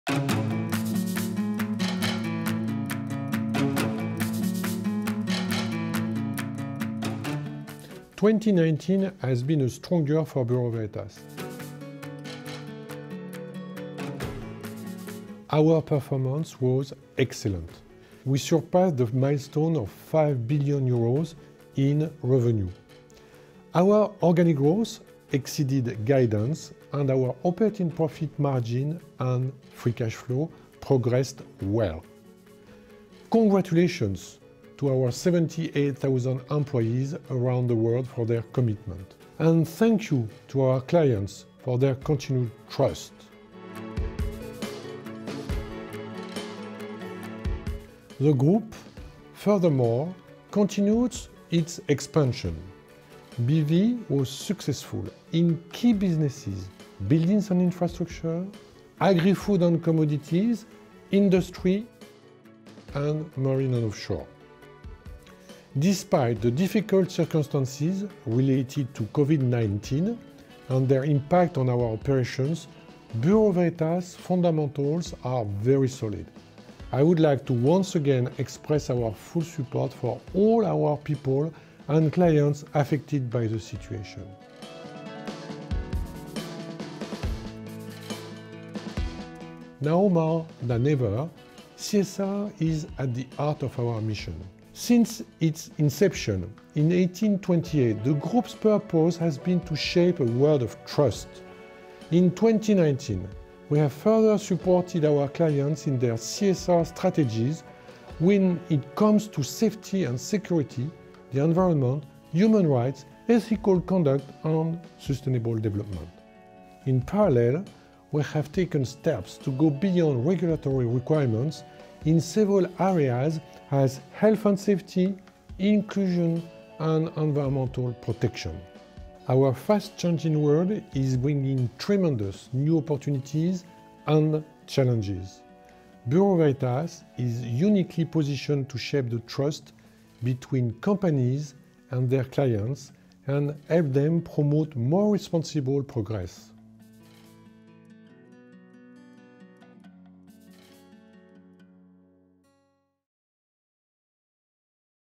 Bureau Veritas Full Year 2019 Results
Bureau Veritas’ strong fundamentals driving an excellent set of 2019 results. They illustrate the extent of the transformation of Bureau Veritas and are a credit to all the teams across the Group.
Discover more in this video.